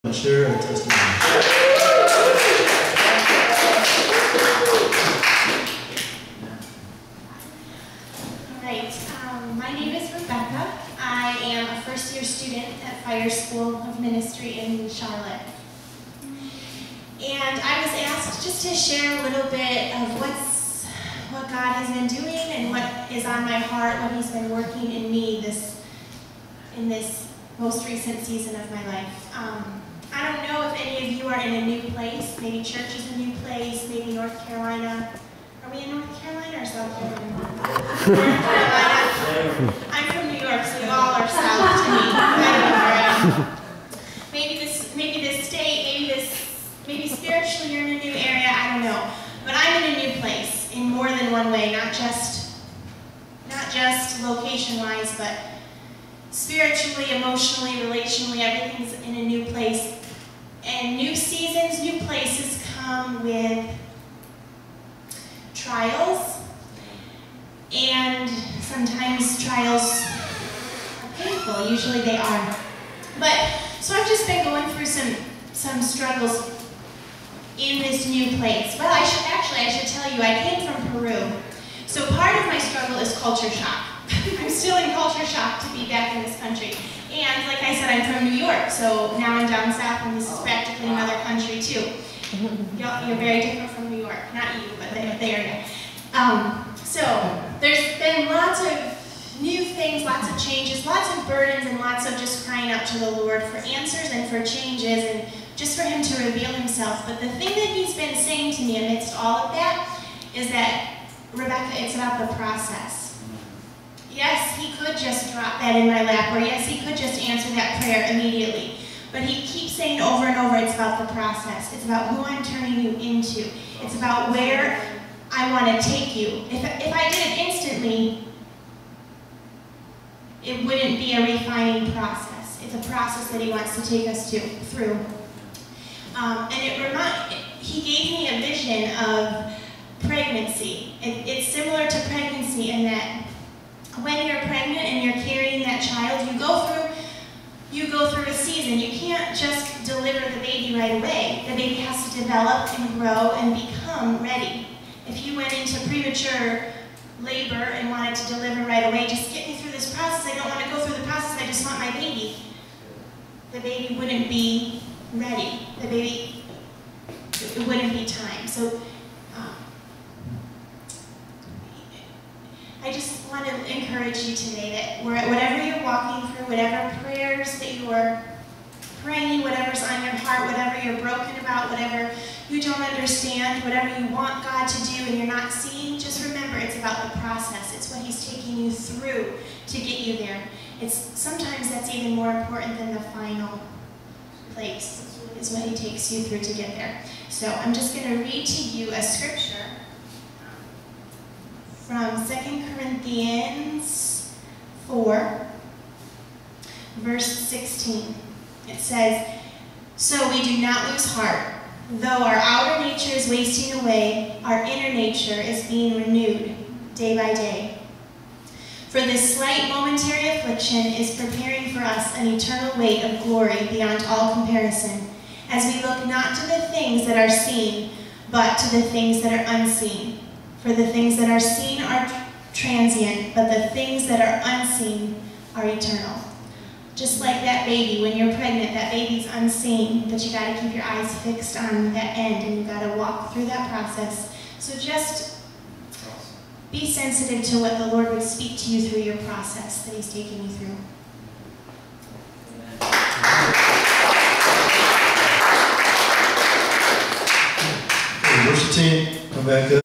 New DestinySept12 FireStudent Testimony-Rebeka.mpg
FIRE School Student - Rebekah shared an incredible testimony at New Destiny Community Church on Sunday, September 12 at 6pm (@ The Club at Westport)